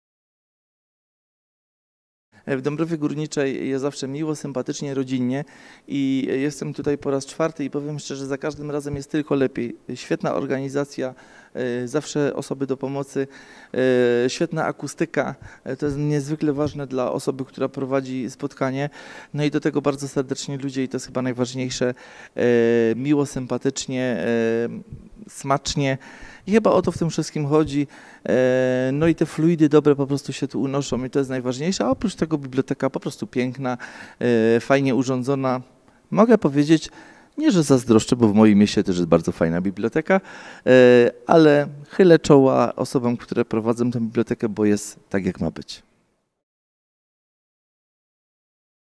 Remigiusz Rączka po spotkaniu w Bibliotece Miejskiej w Dąbrowie Górniczej
Tuż po spotkaniu z czytelnikami Miejskiej Biblioteki Publicznej w Dąbrowie Górniczej i mieszkańcami Zagłębia Remigiusz Rączka podzielił się z nami wrażeniami z tegorocznej przedświątecznej wizyty.

Zapraszamy do obejrzenie naszych innych relacji z wydarzeń i spotkań.
Zachęcamy do odwiedzenia naszej strony gdzie znajdziecie Państwo wszystkie aktualne informacje o wydarzeniach w Bibliotece.